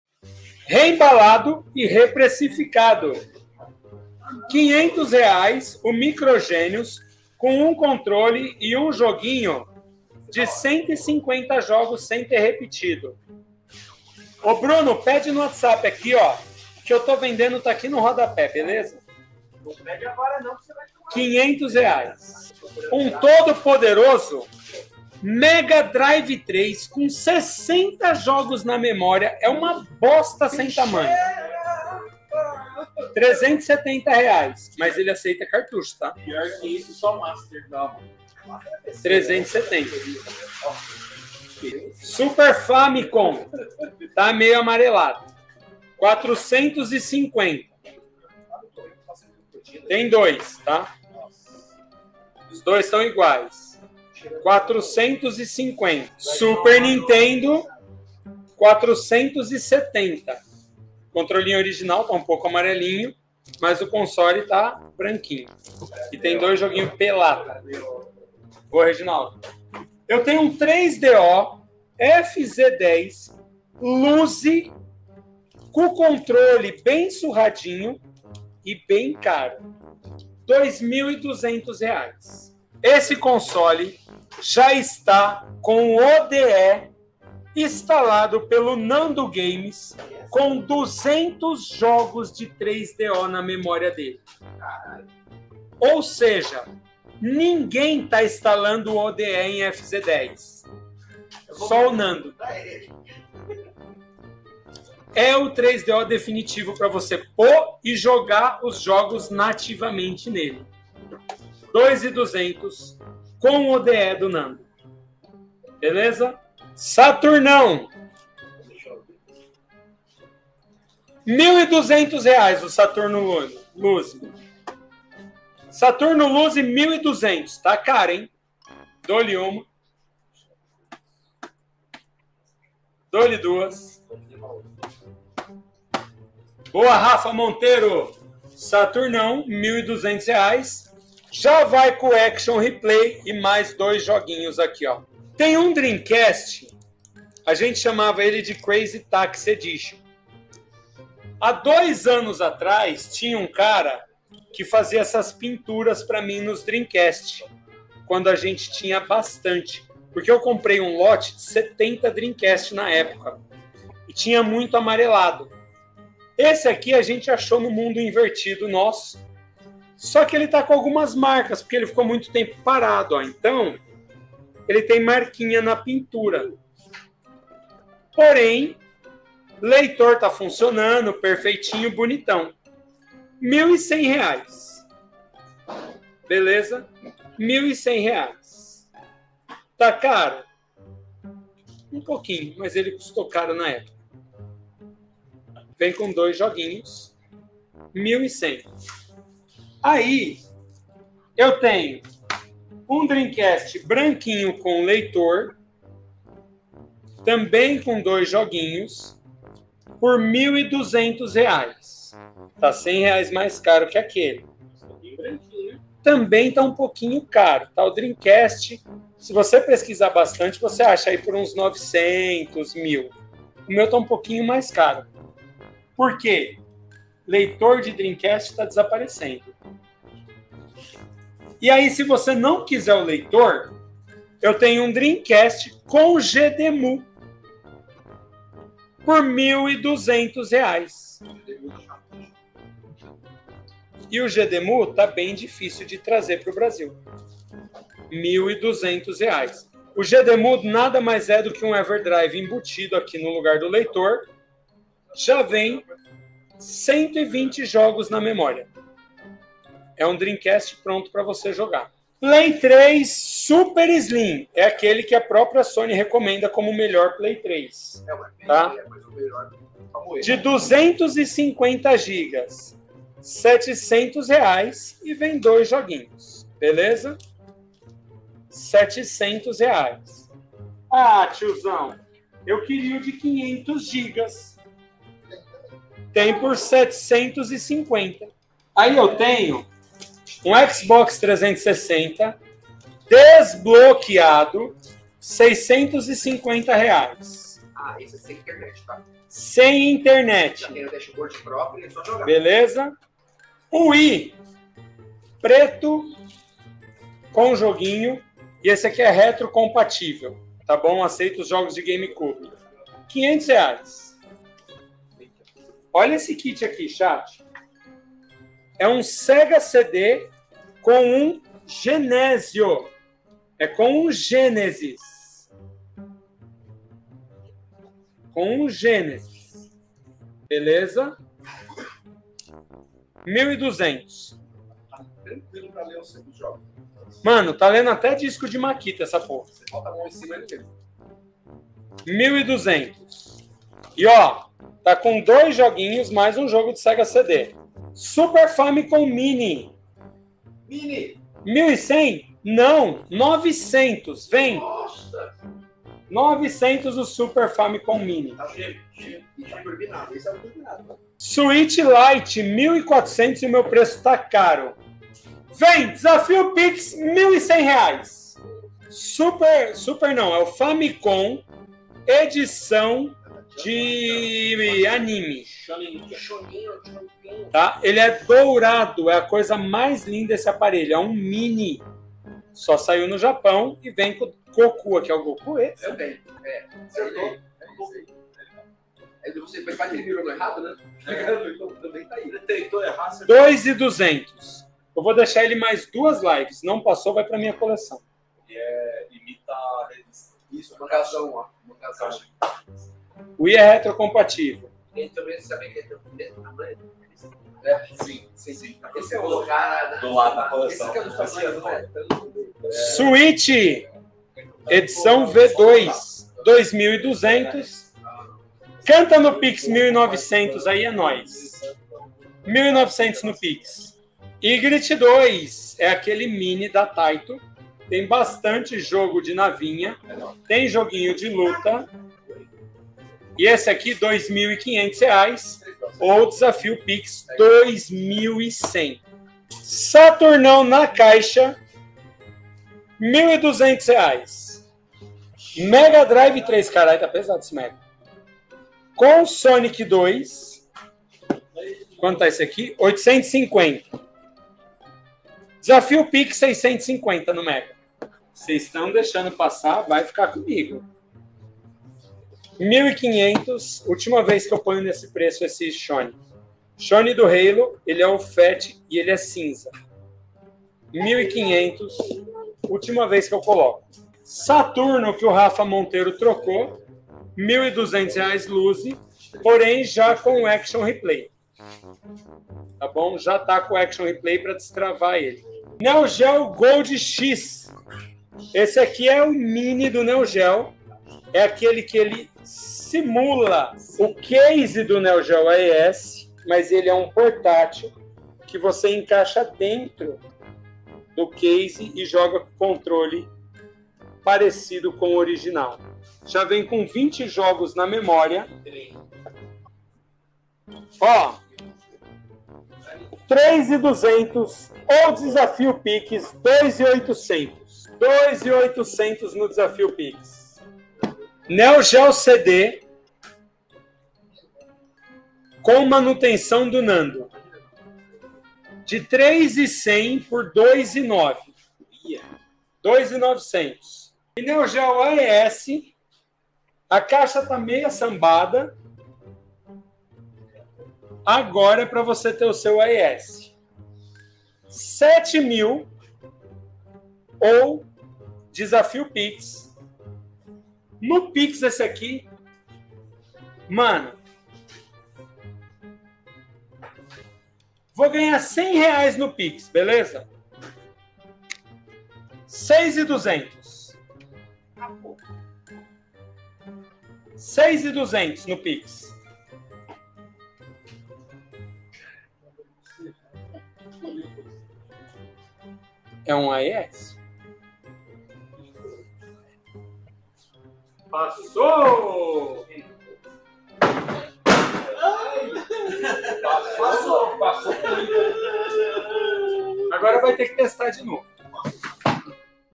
TIOZÃO DERRUBOU O NEO GEO DE $6.200 REAIS NO CHÃO 😭
Tiozão deu mole e derrubou o NEO GEO no CHÃO 😧